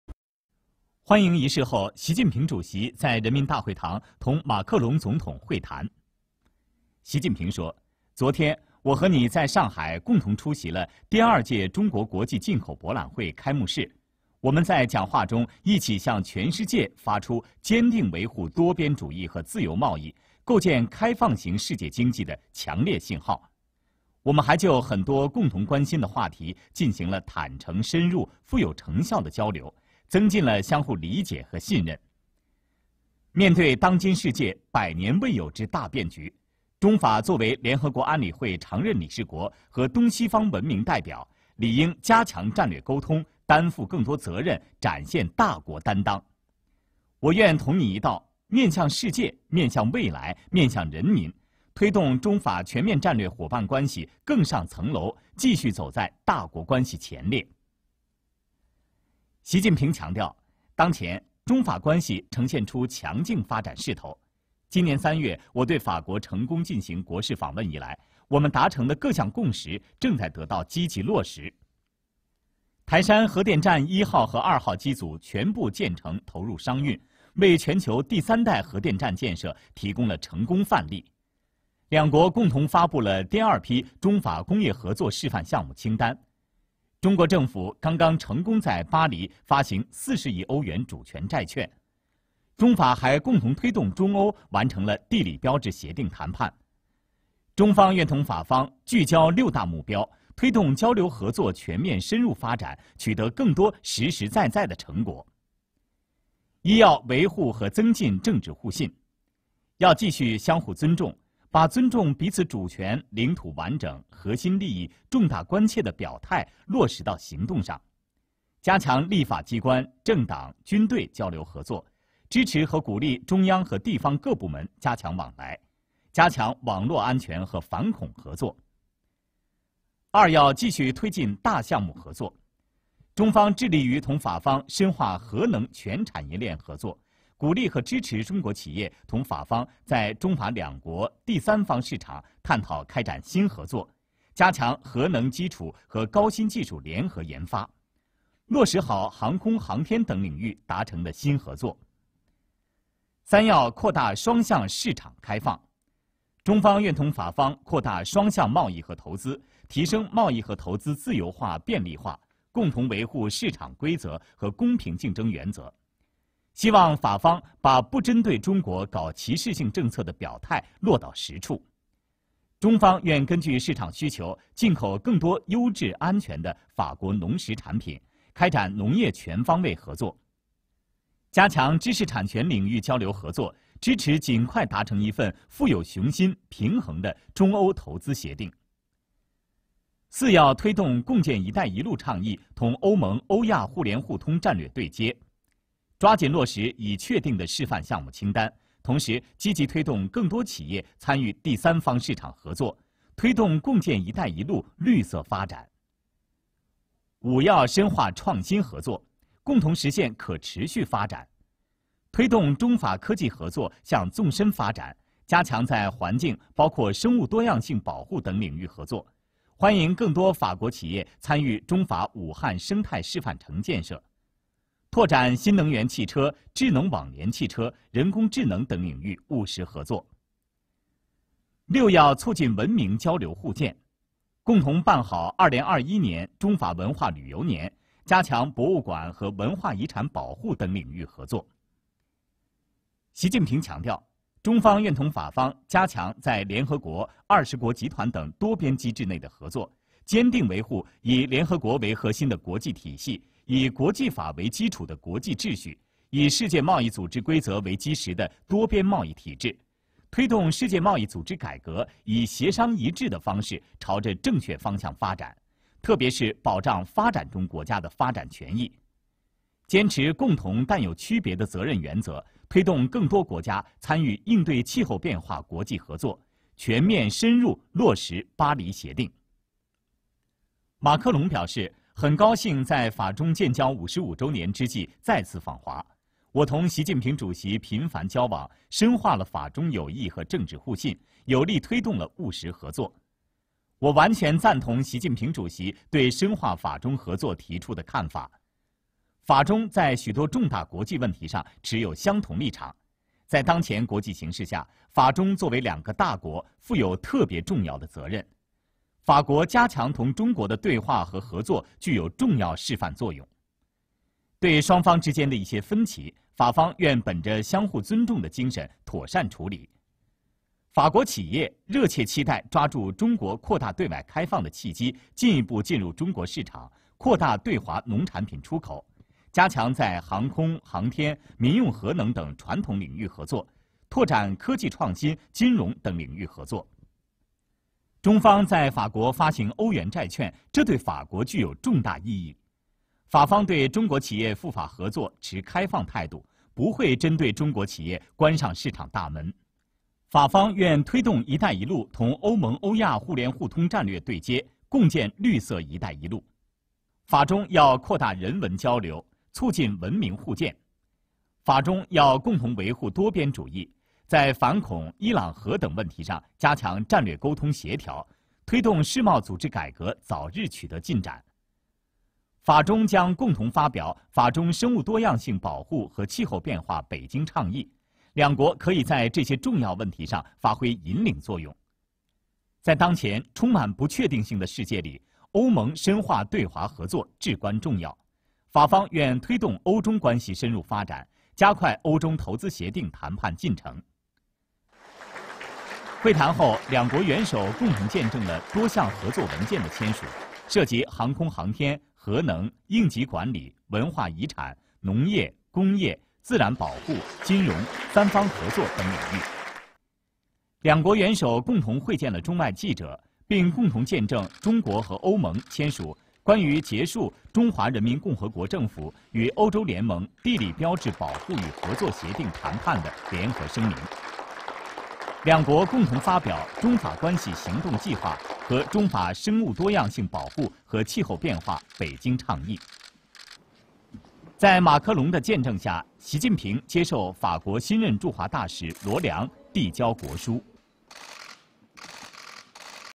[中国新闻] 习近平同法国总统马克龙会谈 | CCTV中文国际
习近平同法国总统马克龙会谈。